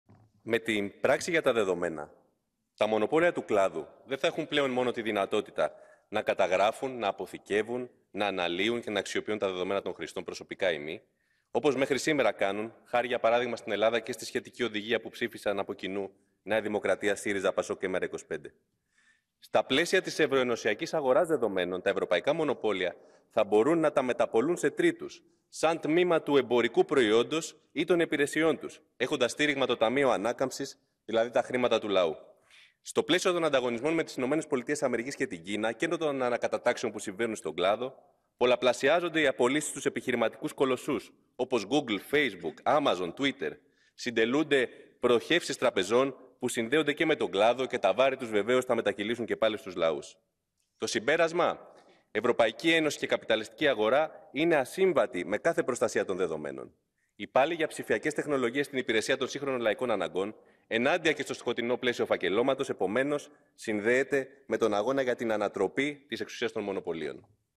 ΕΕ και καπιταλιστική αγορά είναι ασύμβατες με την προστασία των δεδομένων
Στη συζήτηση σχετικά με τον «κανονισμό για τα δεδομένα» στην Ολομέλεια του Ευρωκοινοβουλίου στο Στρασβούργο παρενέβη η Ευρωκοινοβουλευτική Ομάδα του ΚΚΕ.